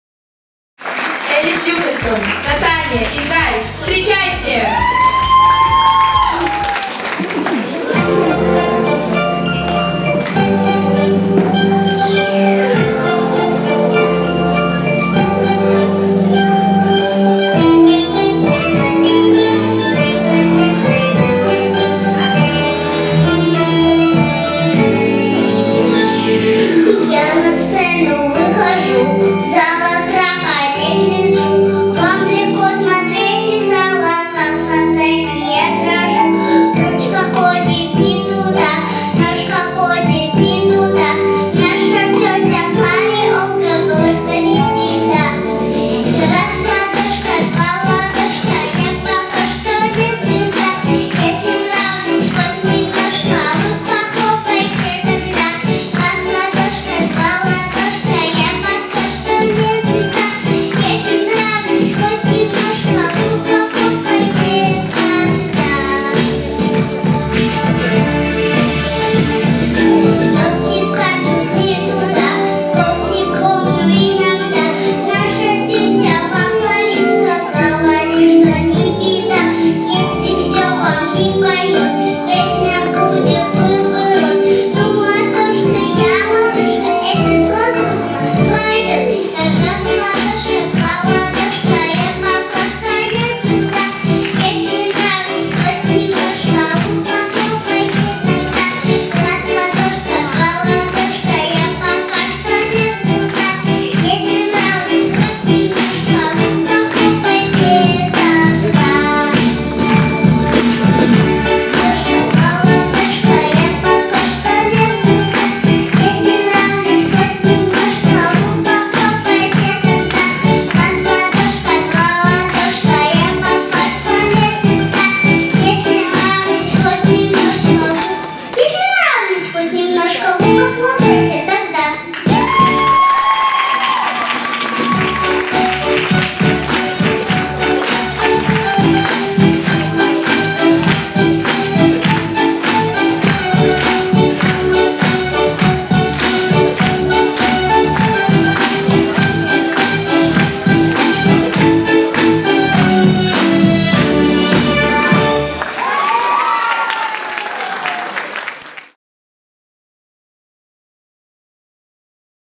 2019 ★ Elis Yukal'son | 4 years | Netanya, Israel ★ Competition ★ «Kaleidoscope» (Israel)
★ Конкурс международного фестиваля-конкурса талантливых детей «Калейдоскоп» (Израиль)
★ קונצרט-גלה של תחרות-פסטיבל בינלאומי של ילדים מוכשרים «קלידוסקופ» (ישראל)